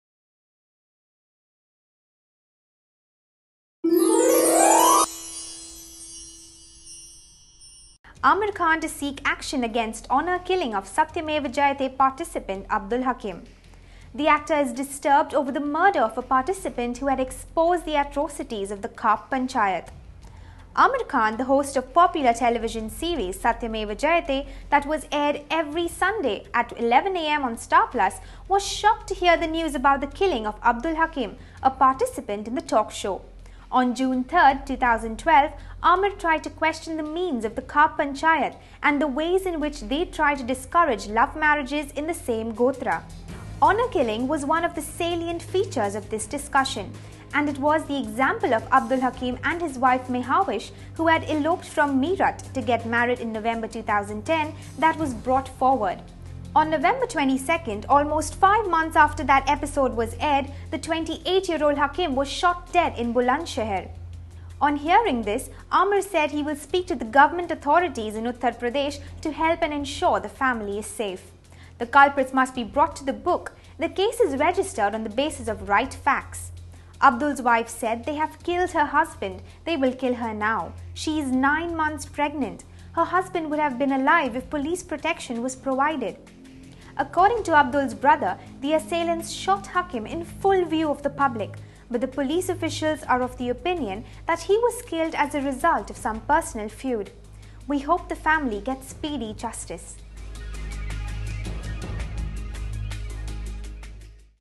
Aamir Khan to seek action against honour killing of Satyamev Jayate participant Abdul Hakim
The actor is disturbed over the murder of a participant who had exposed the atrocities of the Khap panchayat.Aamir Khan, the host of popular television series Satyamev Jayate that was aired every Sunday at 11am on Star Plus.On hearing this, Aamir said, "Will speak to the government authorities in UP (Uttar Pradesh) to help and ensure the family is safe.